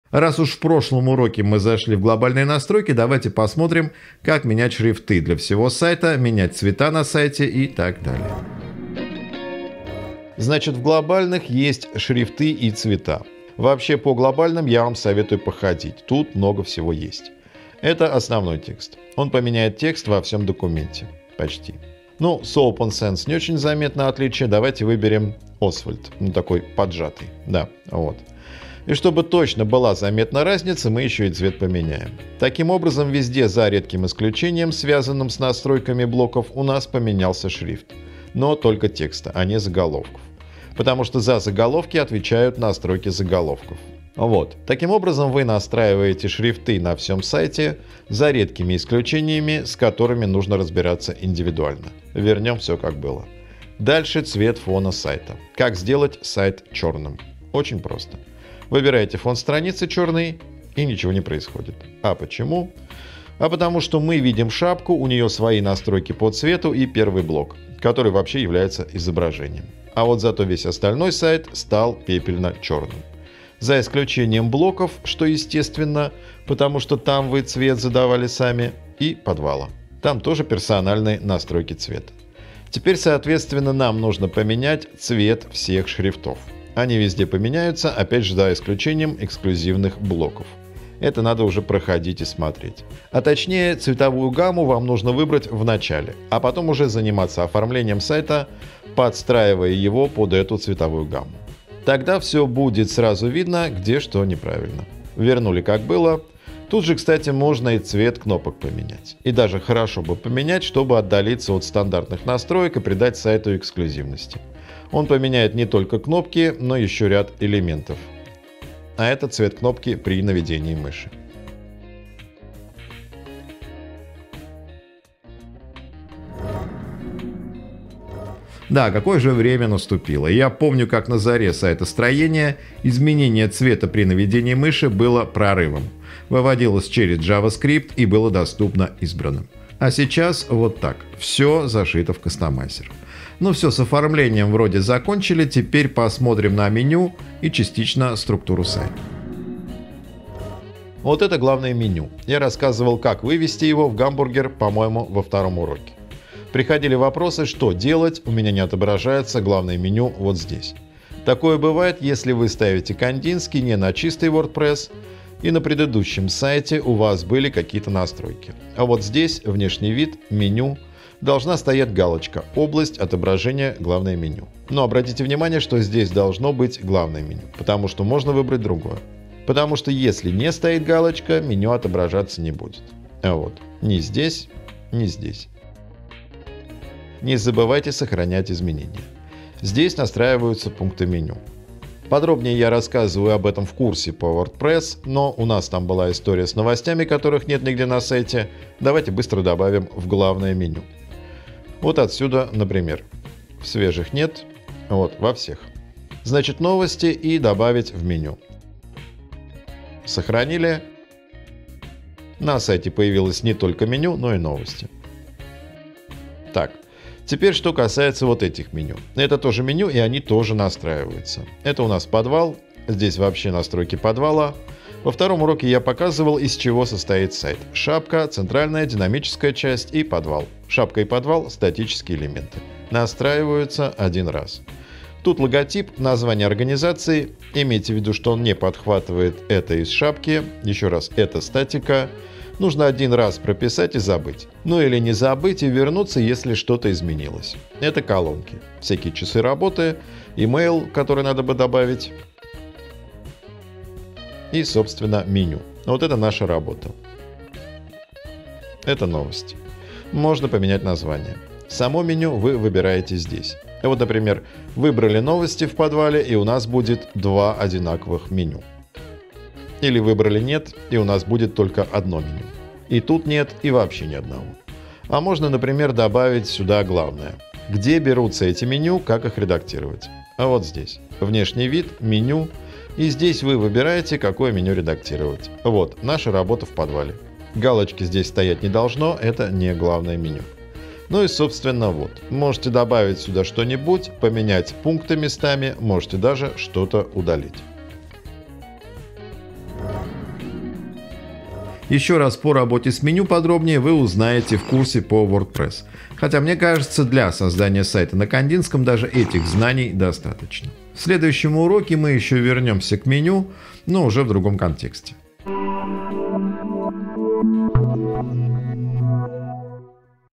Кандинский 2.0 #7: как менять шрифты и цвета на всем сайте. Работа с меню
Разработка Теплицы конструктор сайтов «Кандинский» полностью обновился больше года назад. Мы делали небольшой курс по «Кандинскому», когда он был в бета-версии, а теперь сделали полный курс по итогам года работы. В этом курсе мы постарались ответить на все вопросы, которые присылали по «Кандинскому» в службу поддержки.

«Кандинский» – это бесплатная тема для WordPress, разработанная Теплицей специально для НКО. Это коробочное решение и после установки «Кандинского» вы получаете полностью рабочий сайт со всеми необходимыми плагинами.

Пример выравнивания текста и округления углов изображения через дополнительные стили CSS:

.knd-block .section-title
{margin-left: auto;
margin-right: auto;
text-align: center;}

Body { --knd-image-border-radius:10px }